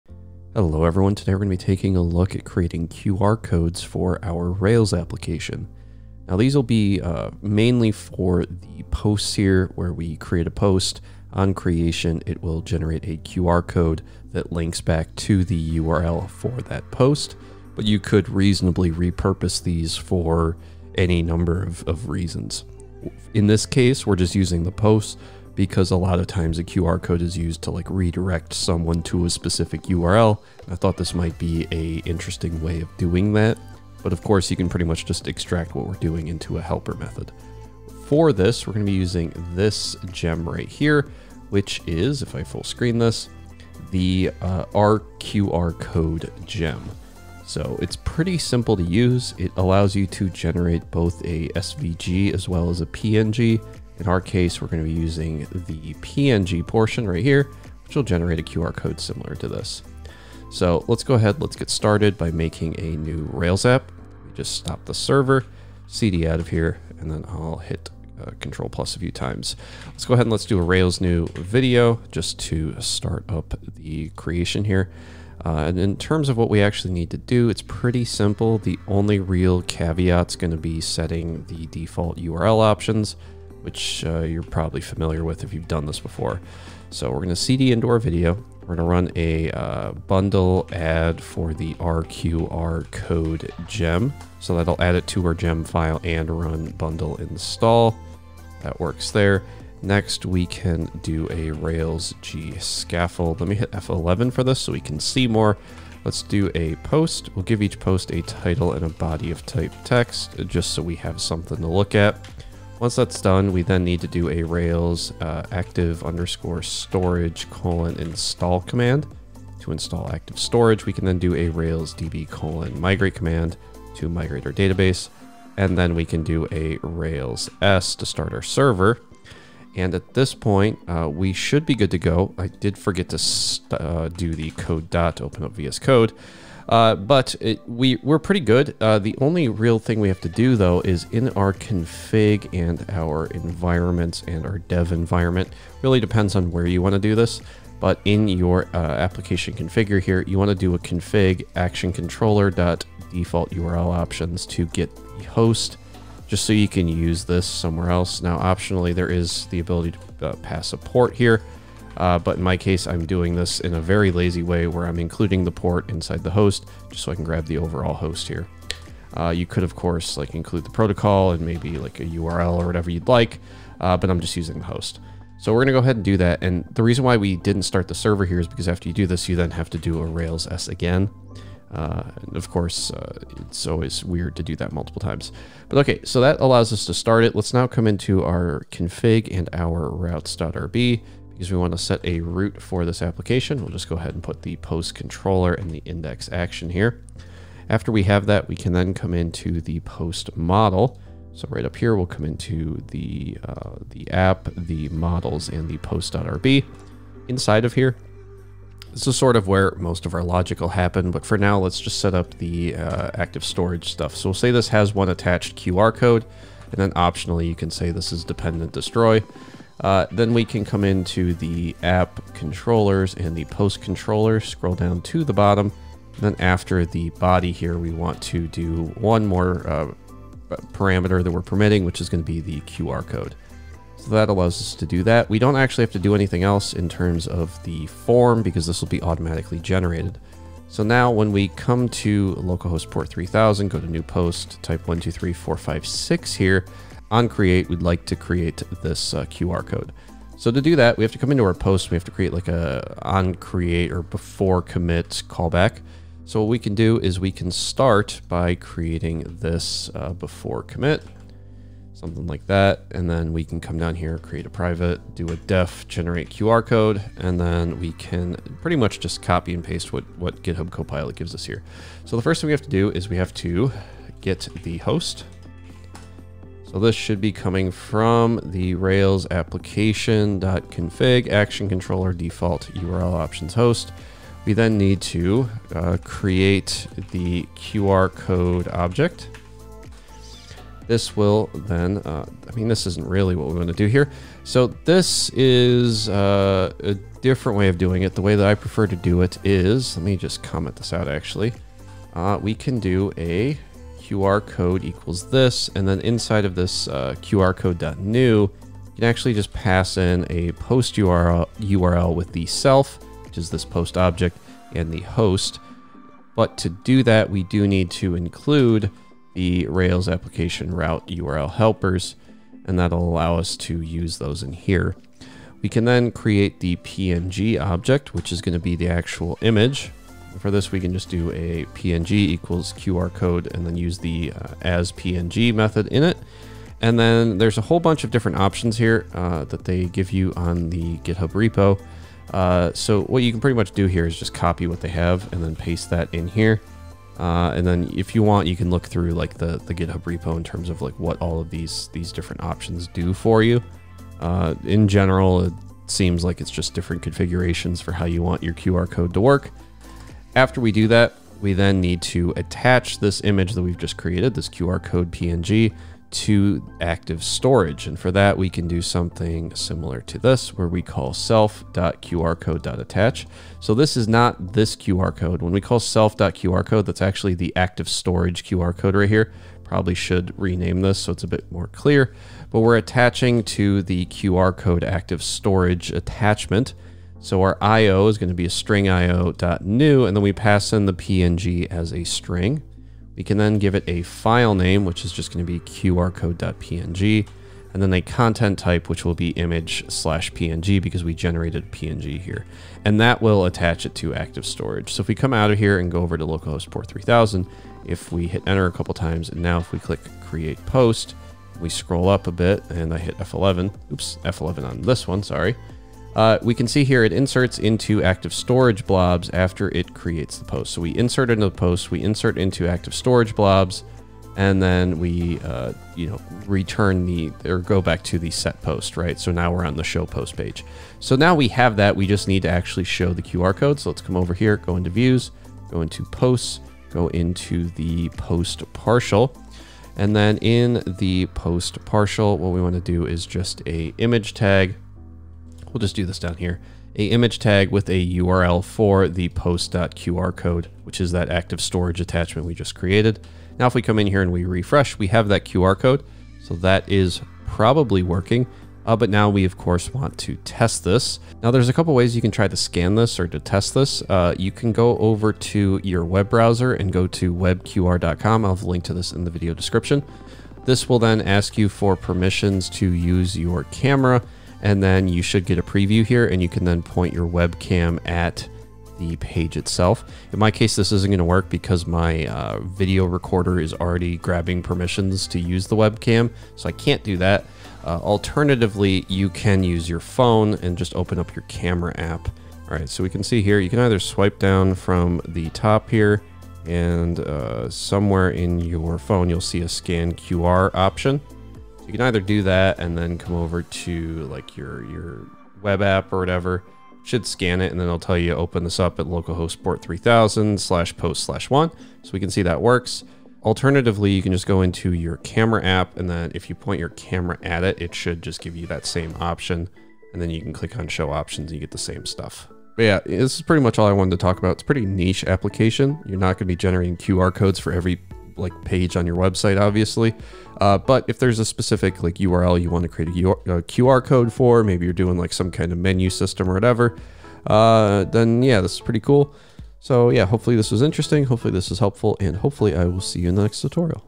Generate QR Codes with Active Storage | Ruby on Rails 7 Tutorial
Creating QR Codes in your Ruby on Rails 7 app is easy with the rqrcode gem and Active Storage.